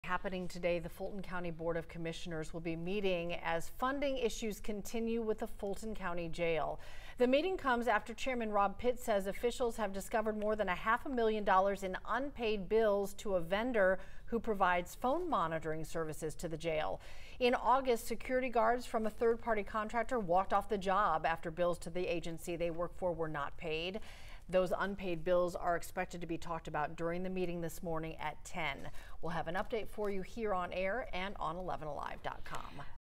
Fulton County commissioners meet today with jail funding still a hot topic
The sheriff's office and Board of Commissioners have been at odds over funding and the payment of contracts.